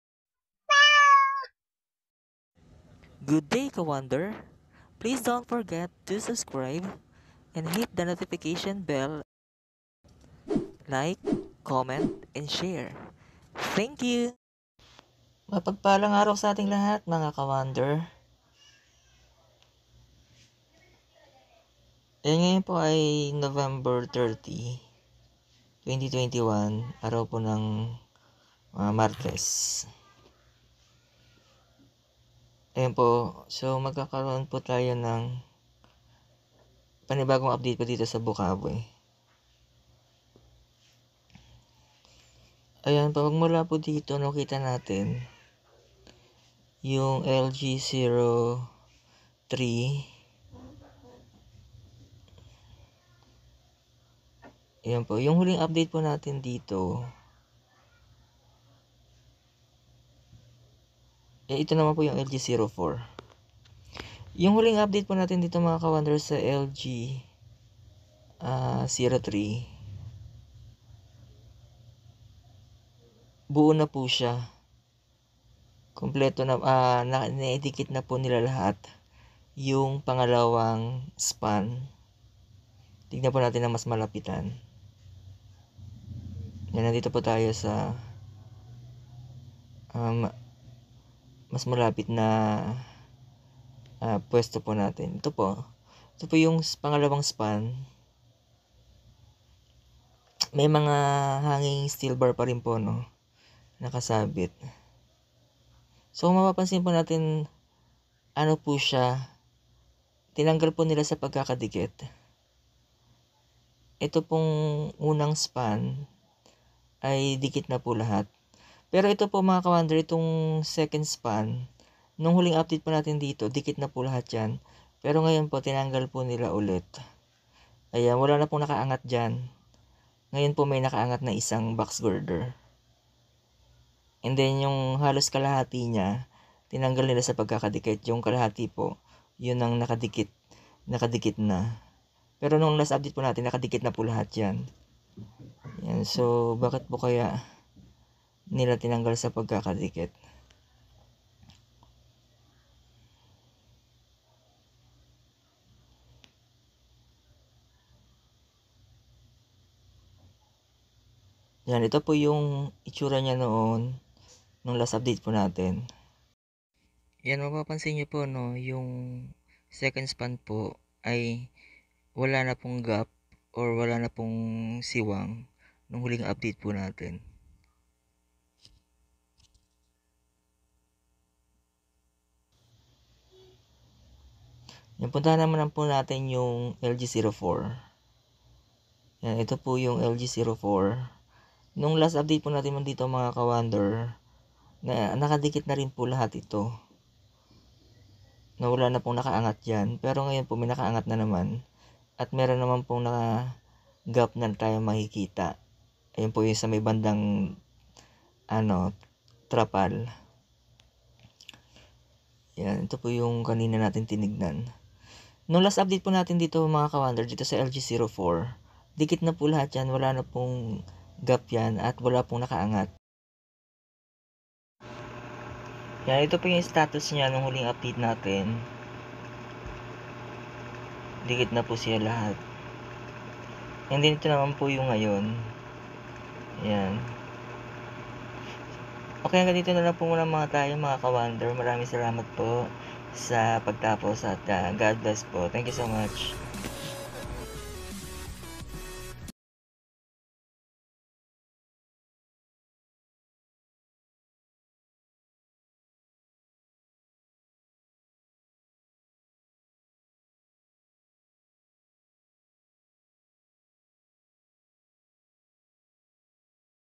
PNR.NSCR. UPDATE BOCAUE.DIKIT NA NOONG LAST UPDATE ITO..BAKIT HINDI NA NGAYON..
PNR.NSCR..Good day ka Wander..welcome to our update here in Bocaue..in our last update it was bonded already the 2nd span viaduct..but now they removed again..thank you Ka Wander.. please.SUBSCRIBE and hit the notification bell if you are new to my channel so that you are updated to the new updates ka Wander..thank you so much and God Bless..